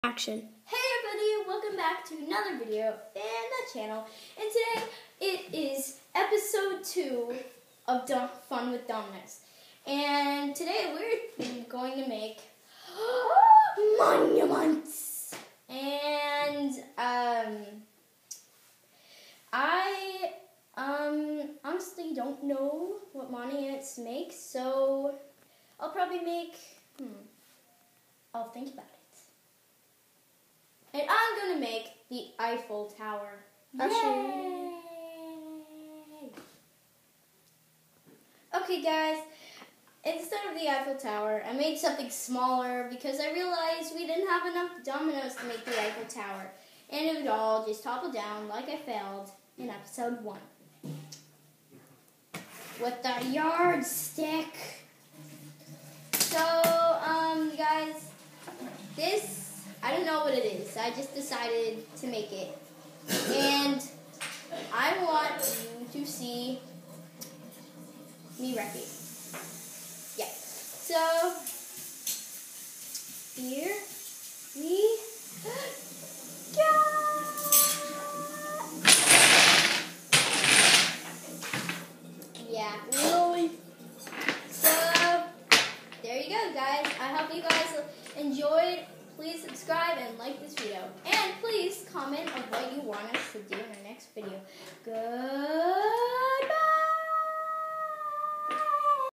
Fun With Dominos! Episode 2 : Monuments
Hey guys! Today I am making monuments with dominos! I hope you liked this video! Goooooooooooood. Byyyyyyyye!